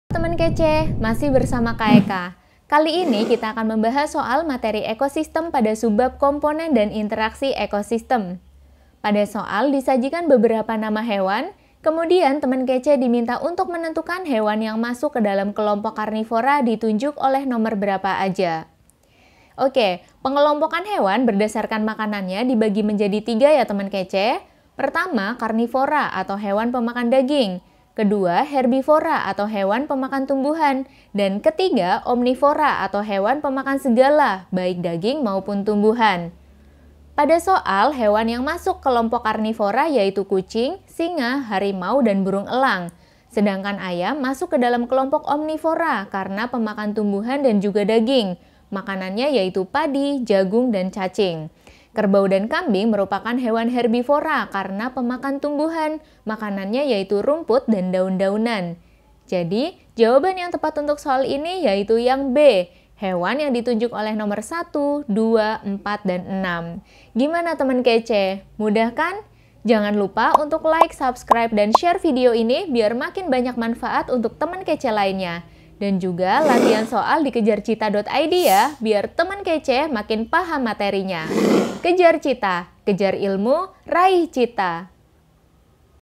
Hewan Mana yang Termasuk Karnivora? | IPA SD
Contoh soal latihan Komponen dan Interaksi Ekosistem (IPA Kelas 5 SD). Halo temen kece, yuk latihan soal gratis di Sudah ada puluhan ribu soal dan berbagai materi untuk temen kece pelajari lhoo...

Nah, belum lama ini kalian sudah belajar tentang ekosistem kan? Hayo, masih ingat ga nih hewan apa aja yang masuk dalam kelompok karnivora?
Kal ini kak eka mau bahas tentang pengelompokan hewan berdasarkan makanannya (untuk menjawab soal). Stay tune ya temen kece!
Salam!